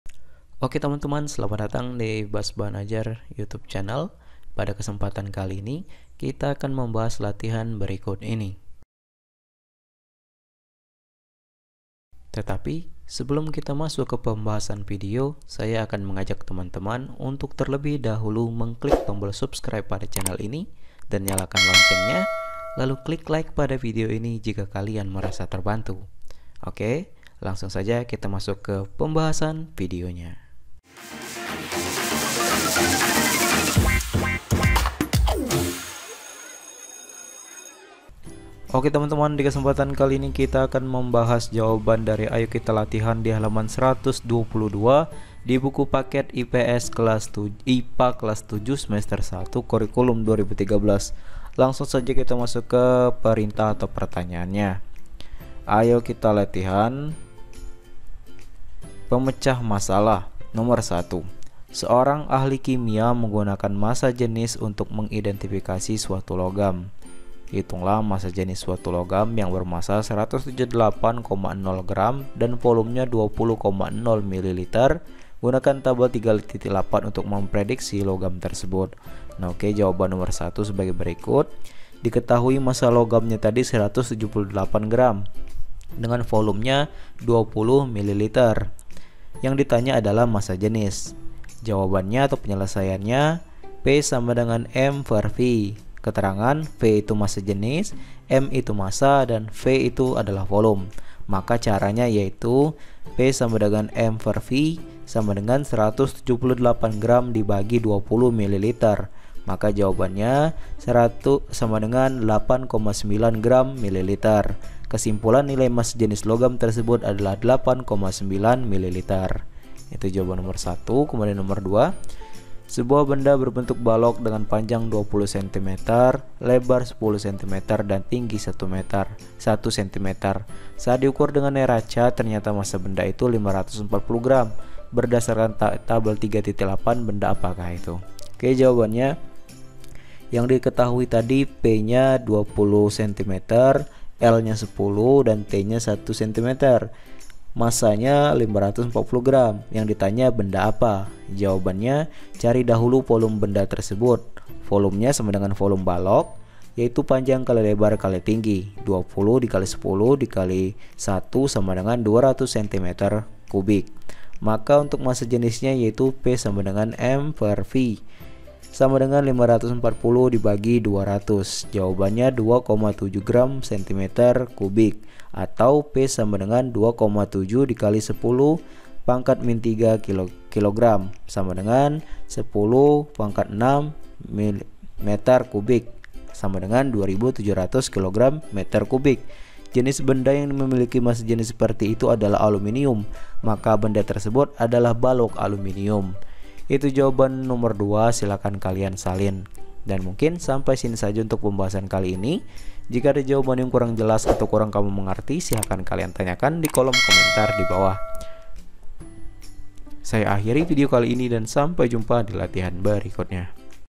Kunci Jawaban IPA Kelas 7 Halaman 122 Ayo Kita Latihan
Berikut ini merupakan pembahasan kunci jawaban Buku Ilmu Pengetahuan Alam untuk Kelas 7 halaman 122. Pembahasan kali ini kita akan bahas latihan yang ada pada buku paket IPA Apakah yang dimaksud dengan massa jenis merupakan salah satu karakteristik (ciri) suatu zat? halaman 122 Buku siswa untuk Semester 1 Kelas VII SMP/MTS. Semoga dengan adanya pembahasan kunci jawaban Pilihan Ganda (PG) dan juga Esaay Bab 3 Klasifikasi Materi dan Perubahannya Kelas 7 ini, kalian bisa menjadi lebih giat untuk belajar. Kunci jawaban ini diperuntukkan untuk para pelajar yang sedang mengerjakan tugas Kurikulum 2013 (K13). Kunci Jawaban Ayo Kita Latihan Hal 122 IPA Kls 7

Jawaban IPA Kelas 7 Halaman 122 Ayo Kita Latihan

Ayo Kita Latihan
Apakah yang dimaksud dengan massa jenis merupakan salah satu karakteristik (ciri) suatu zat?

Pemecahan Masalah
1. Seorang ahli kimia menggunakan massa jenis untuk mengidentifikasi suatu logam. Hitunglah massa jenis suatu logam yang bermassa 178,0 gram dan volumenya 20,0 mililiter. Gunakan Tabel 3.8 untuk memprediksi logam tersebut.
2. Sebuah benda berbentuk balok dengan panjang 20 cm lebar 10 cm dan tingginya 1 cm. Saat diukur dengan neraca, ternyata massa benda itu 540 g. Berdasarkan Tabel 3.8, benda apakah itu?

Berpikir Kritis
Apakah massa jenis suatu zat akan berubah jika wujud zat itu berubah? Diskusikan dengan temanmu.